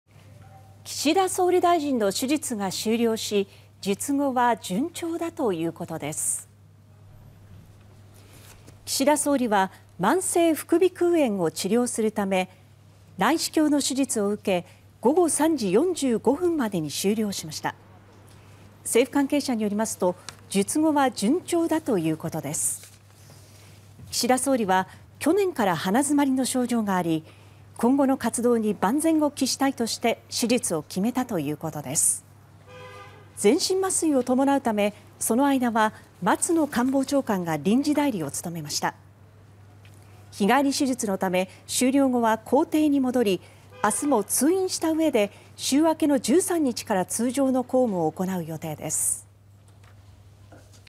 岸田総理「慢性副鼻腔炎」手術が終了 術後も順調(2023年2月11日)
岸田総理大臣の手術が終了し、術後は順調だということです。

　岸田総理は「慢性副鼻腔（びくう）炎」を治療するため、内視鏡の手術を受け、11日午後3時45分までに終了しました。

　政府関係者によりますと、術後は順調だということです。

　岸田総理は、去年から鼻詰まりの症状があり「今後の活動に万全を期したい」として手術を決めたということです。

　全身麻酔を伴うため、その間は松野官房長官が臨時代理を務めました。

　日帰り手術のため終了後は公邸に戻り、12日も通院したうえで週明けの13日から通常の公務を行う予定です。